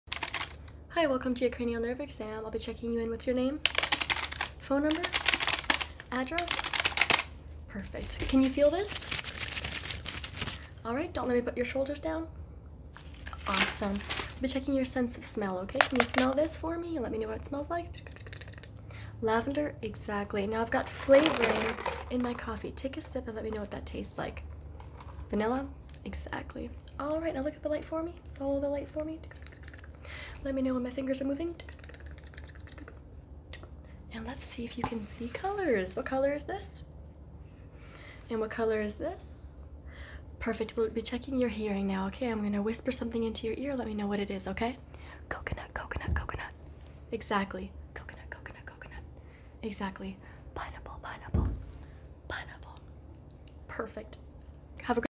ASMR 1 MINUTE CRANIAL NERVE EXAM (1 MINUTE ASMR)
In this video I test your five senses! Touch, eyesight, smell, hearing, and taste.